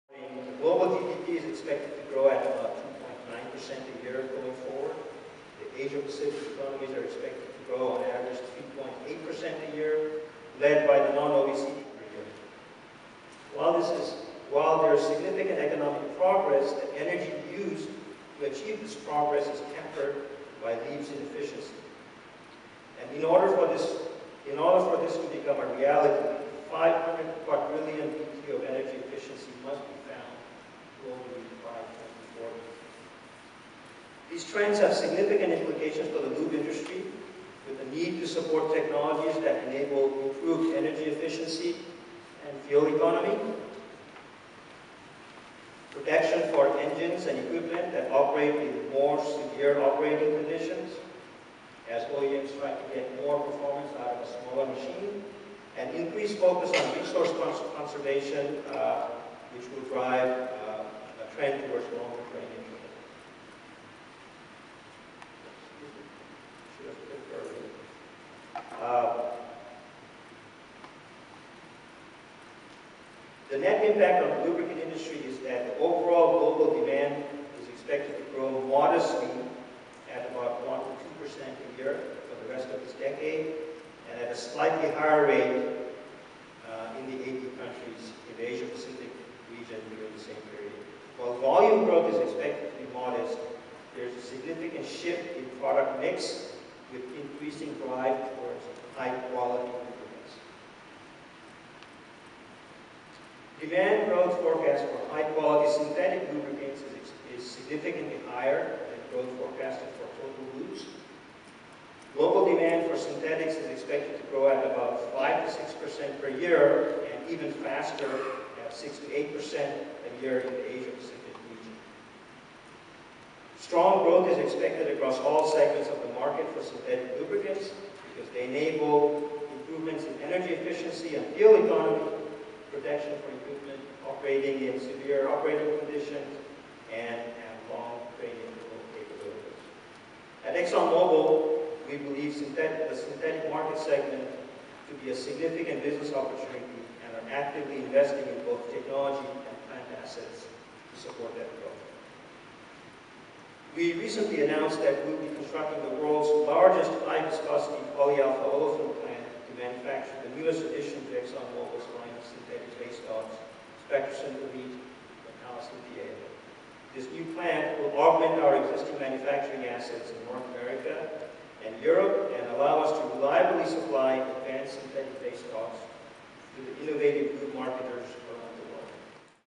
6th Asia-Pacific Base Oil, Lubricant & Grease Conference: Habib Quazi
6th Asia-Pacific Base Oil, Lubricant & Grease Conference
Global Energy Outlook and Implications

Habib Quazi
Former Vice President
Synthetic (currently Marketing Manager for Global Aromatics Business)
ExxonMobil Chemical

Abstract

Mega trends driving growing global energy demand present many challenges and opportunities for the lubricant industry requiring formulations that offer higher fuel economy, energy efficiency and protection for equipment operating in more severe conditions. As a result, demand for high-performing lubricants will continue to increase, especially in the Asia Pacific markets. In particular, synthetic lubricants are well positioned to leverage the global trends to create value for the world and lube marketers.

Habib Quazi was vice president of Synthetics at ExxonMobil Chemical Co. from 2009 to 2011. In this position, Habib's responsibilities included oversight of ExxonMobil Chemical's worldwide synthetic fluids business. Prior to this position, he was Asia Pacific marketing manager for ExxonMobil Lubricants & Specialties, responsible for the development and implementation of marketing plans for automotive and industrial lubricants across the Asia Pacific region. In 2012, Habib was appointed marketing manager for ExxonMobil Chemical's global aromatics business based in Singapore.

Habib has a Bachelor's degree in Mechanical Engineering from Rensselaer Polytechnic Institute and a Master's in Business Administration from the Kellogg School of Management, Northwestern University. He joined ExxonMobil in 1990 and has held technical and management positions at Exxon Research & Engineering, Exxon Company Intl., ExxonMobil Refining & Supply, ExxonMobil Fuels Marketing, ExxonMobil Lubricants & Specialties and ExxonMobil Chemical.